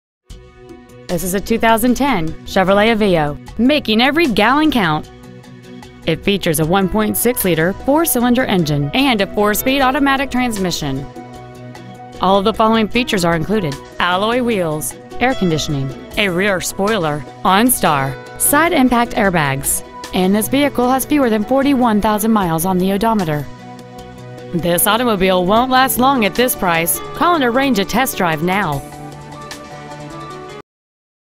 Preowned 2010 Chevrolet Aveo Stockton CA
Year : 2010
 Make : Chevrolet
 Model : Aveo
 Engine : 4-cyl 1.6 liter
 Trans . : automatic
 Exterior : yellow
 Miles : 40,493